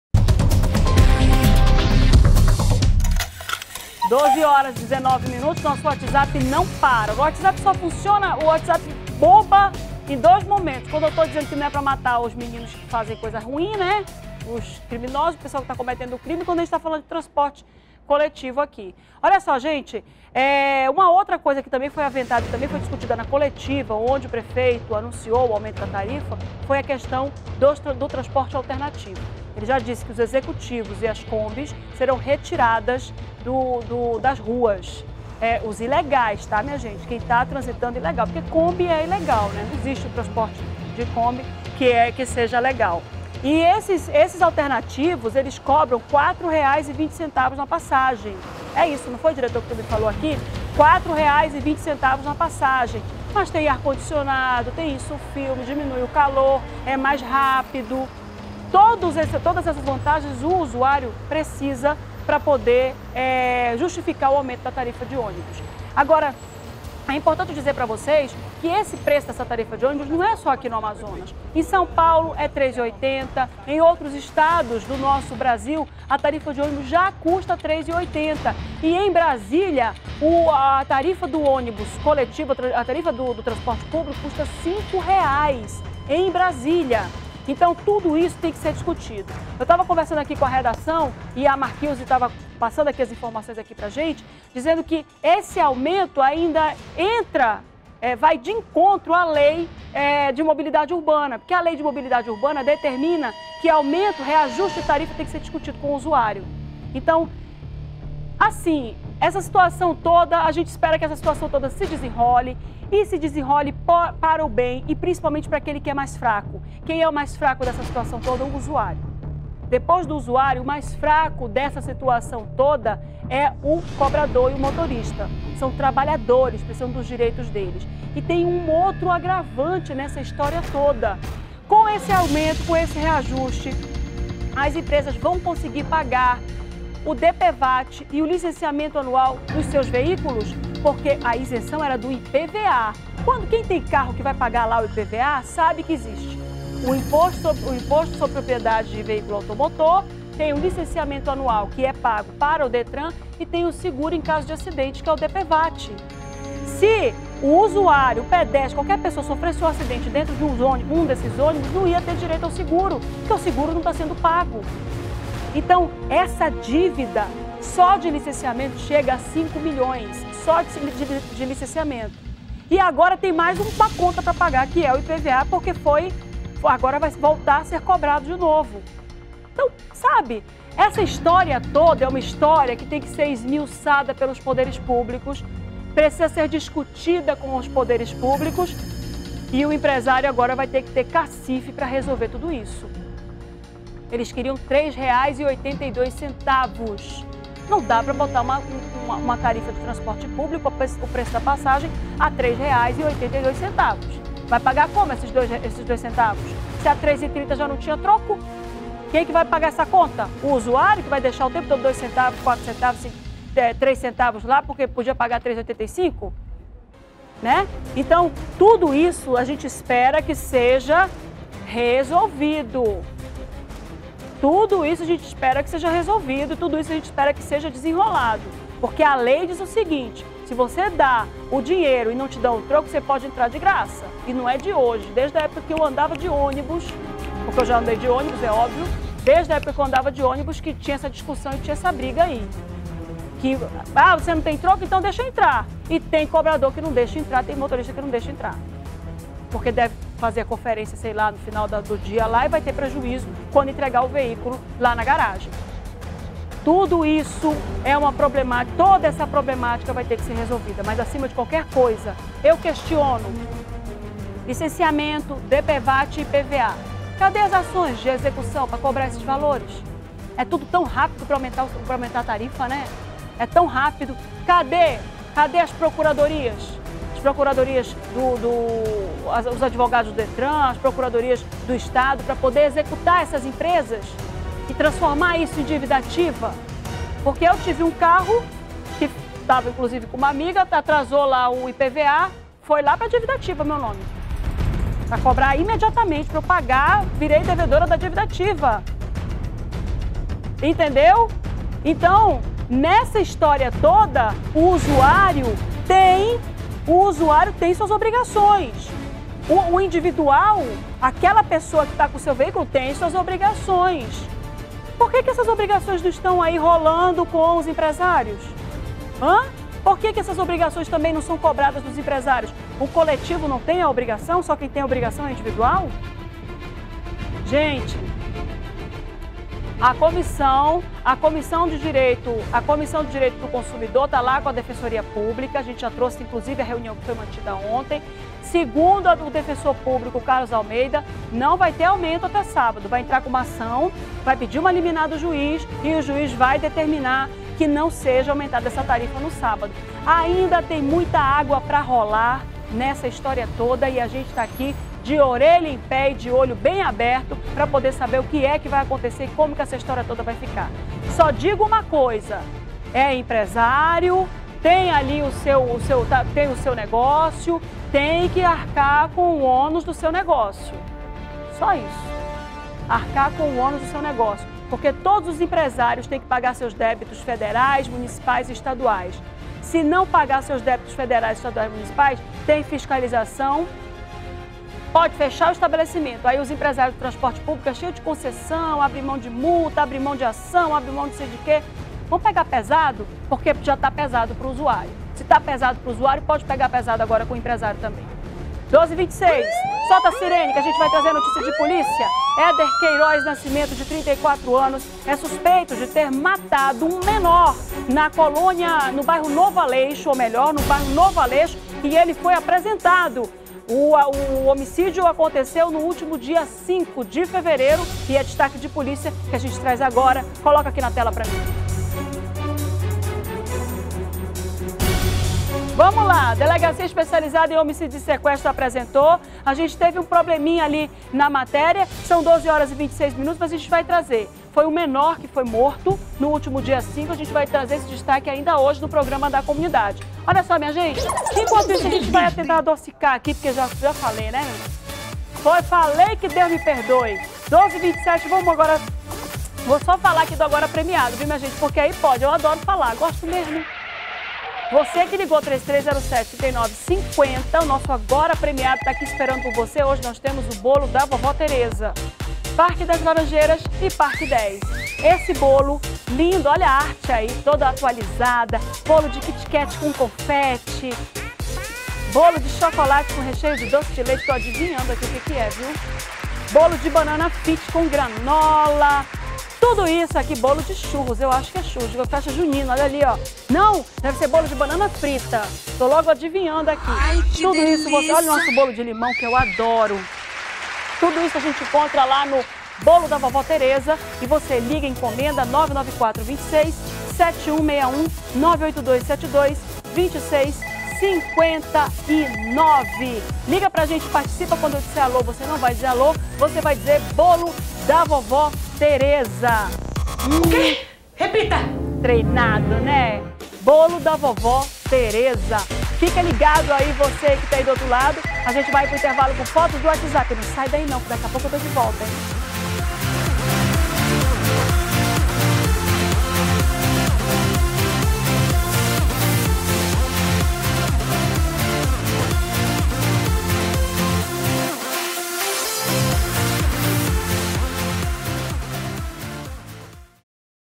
Prefeitura de Manaus deve coibir circulação de executios e alternativos irregulares
04º BLOCO AGORA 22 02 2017 - Prefeitura  de Manaus deve coibir circulação de executios e alternativos irregulares